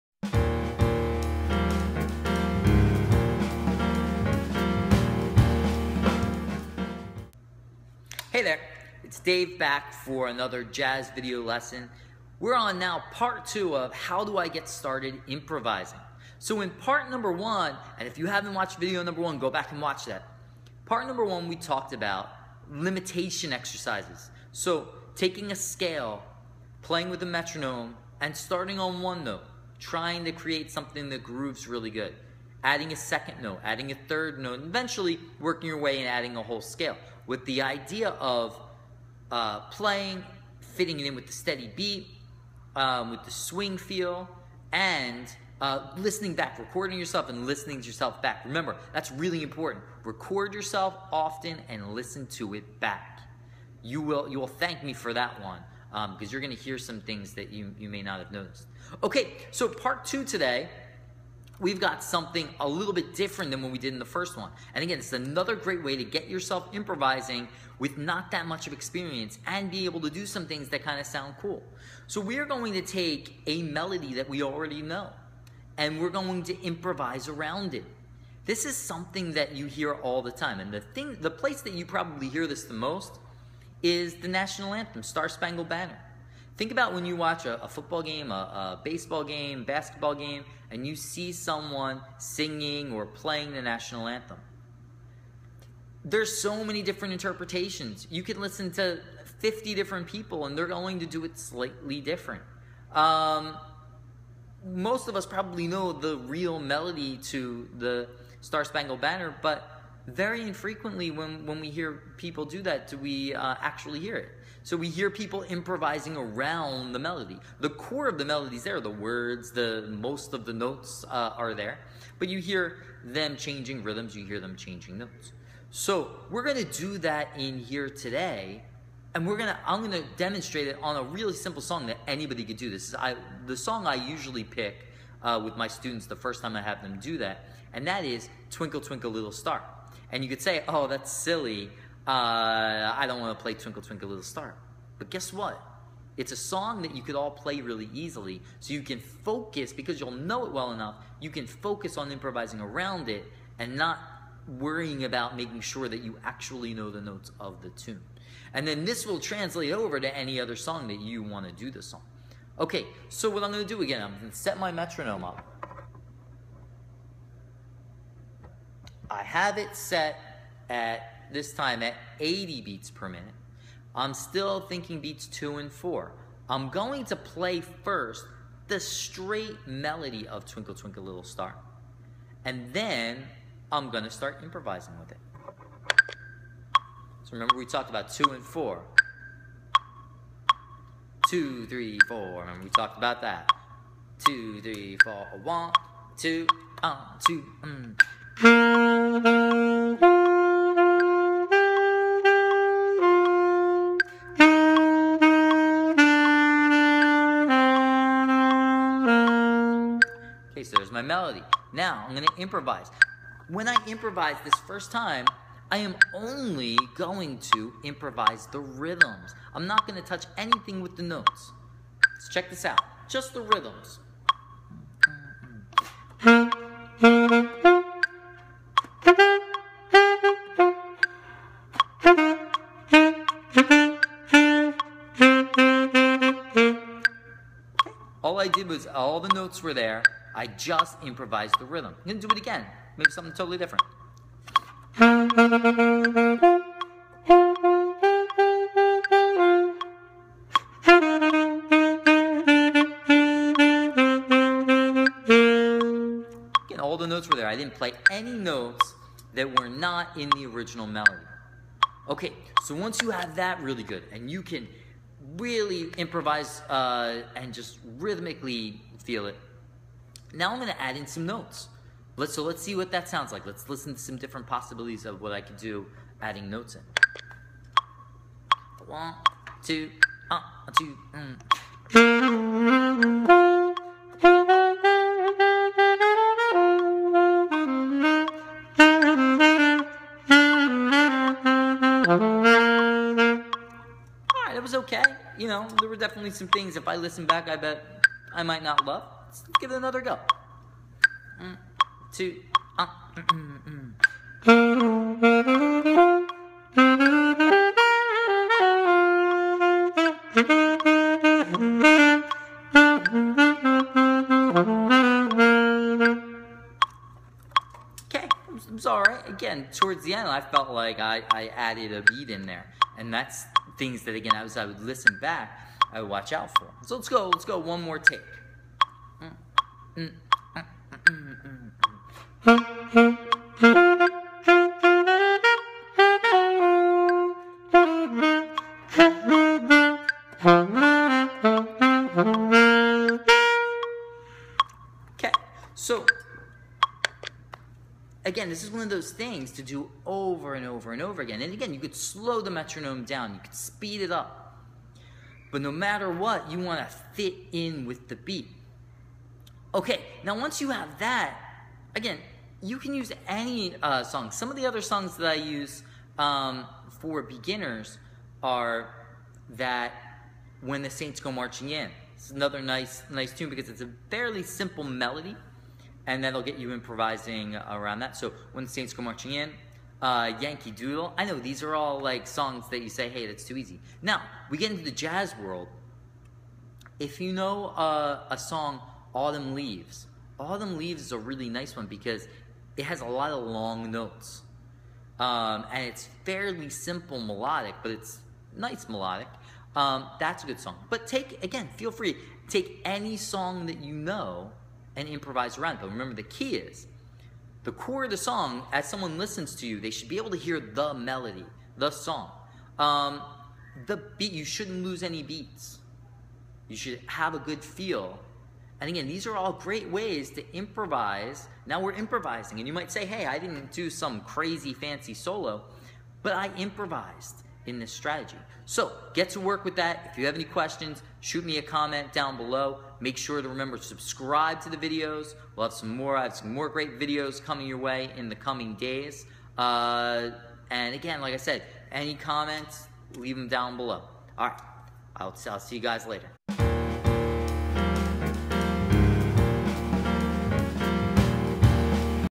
Getting Started Part 2
In part 2 of the getting started with improvising series, Dave talks about improvising around a melody you are already familiar with